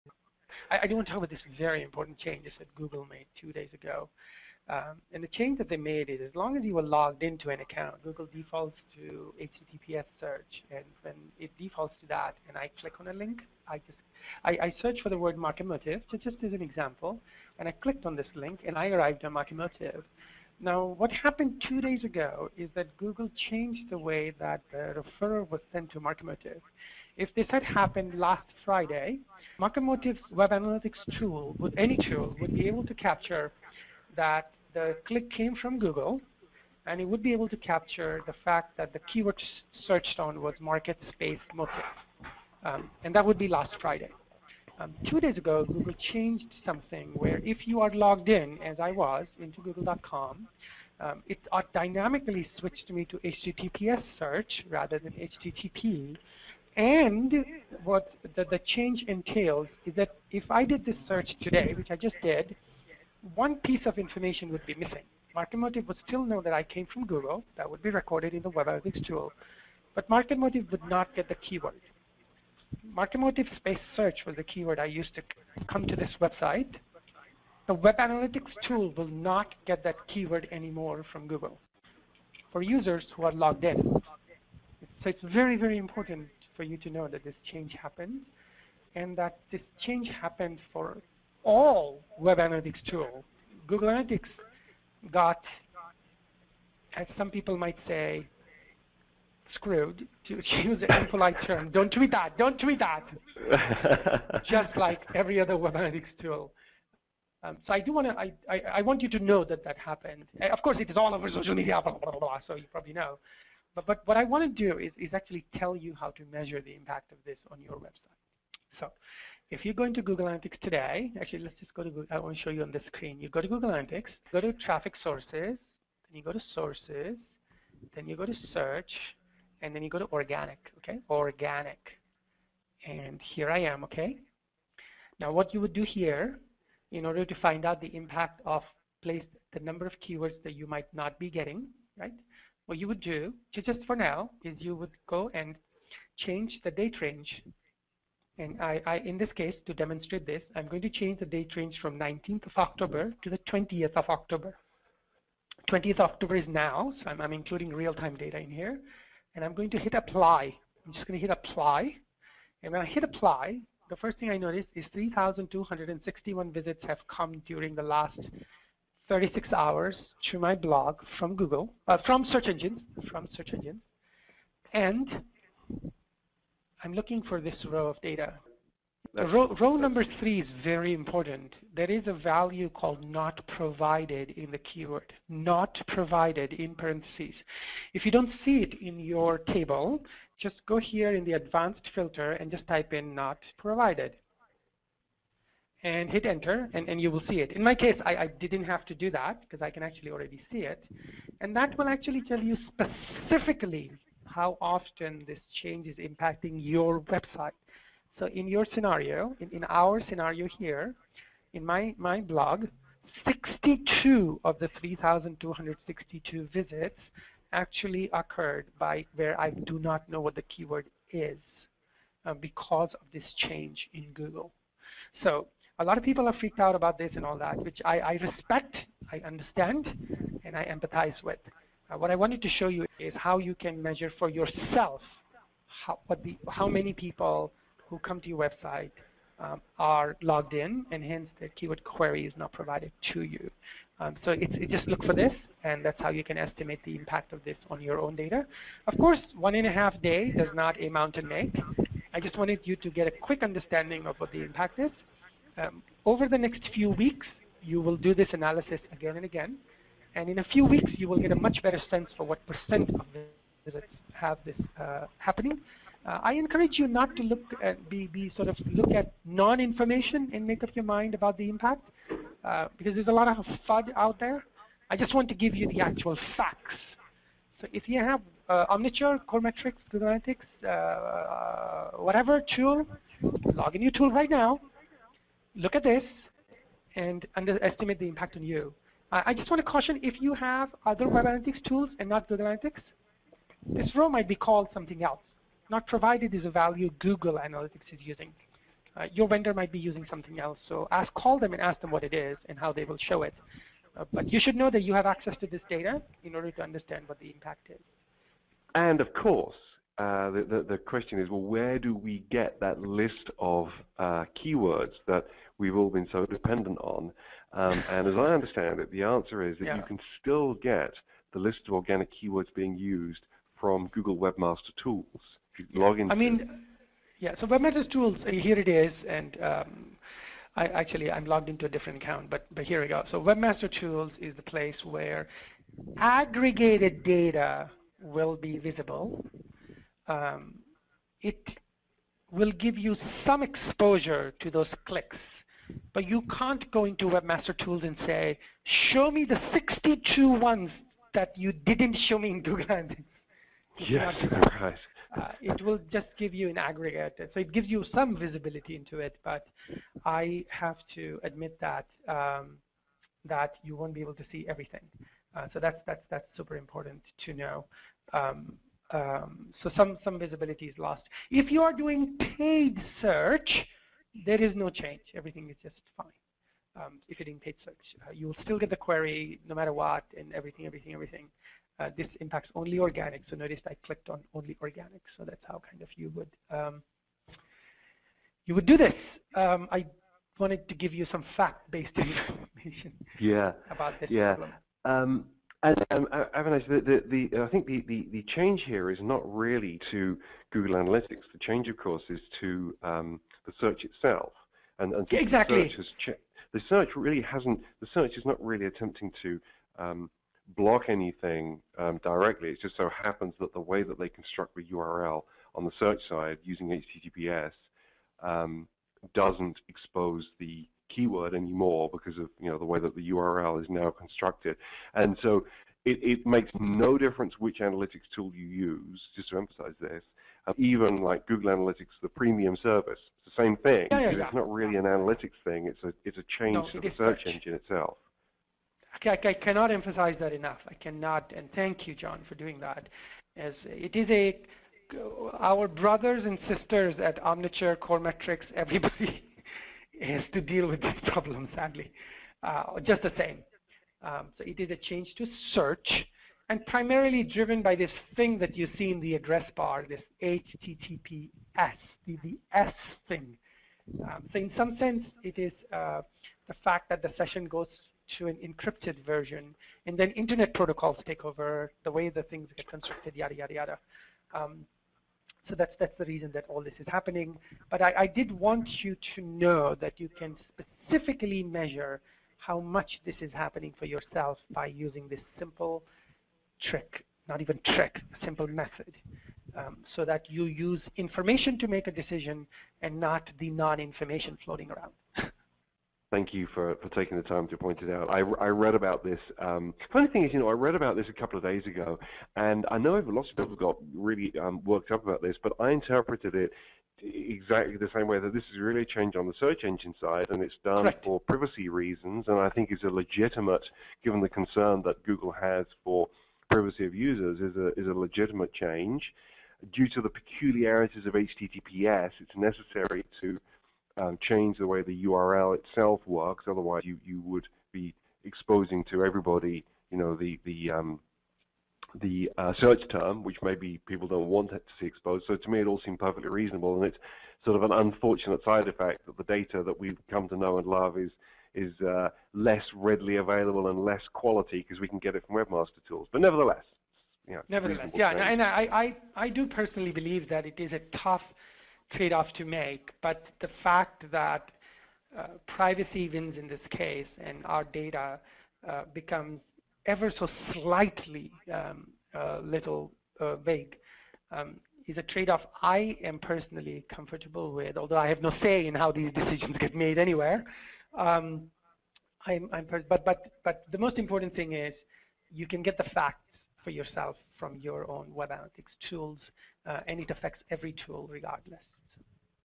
Avinash Kaushik - Google's SSL Update Impact
Google switched search for their logged-in users to SSL (HTTPS). At first, this doesn't sound like much of an issue, but because of the way URLs are constructed when searching on SSL, analytics tools can't tract the keywords used.